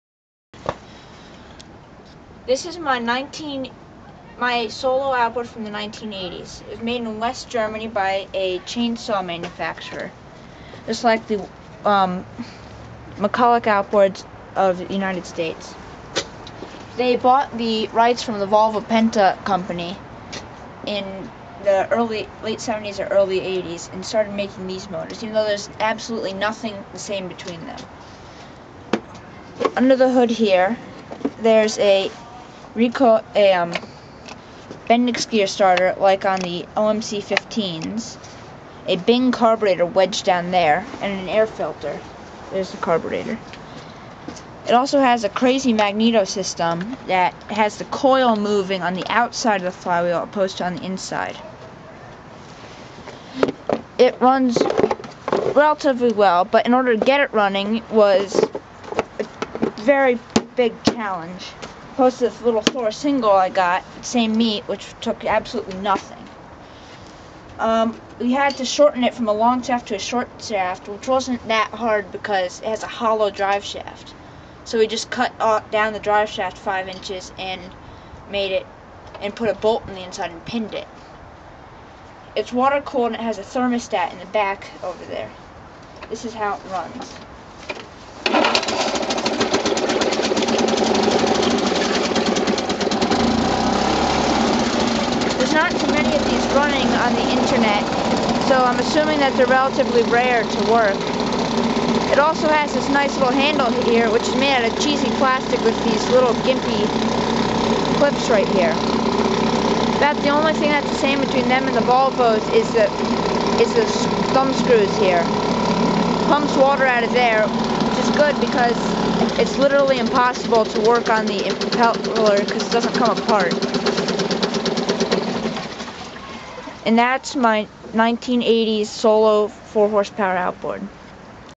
Harry's German Solo 40 (6hp) Running
Here's a short video of Harry's German Solo 40 from the 1980s running. It is a 6hp motor, I have no idea why it is called a 40! Definitely a contender for the least serviceable outboard ever made, it actually makes the low-profile Scotts from the 1960s look great! Dad helped Harry try to check and replace the impeller, we never were able to figure out how to get the housing apart. The tiller is another part that needed attention due to a stripped nylon gear but was also non-serviceable. On the plus side it runs well and is pretty quiet and smooth - we would not suggest ever trying to use one of these as a daily fishing motor.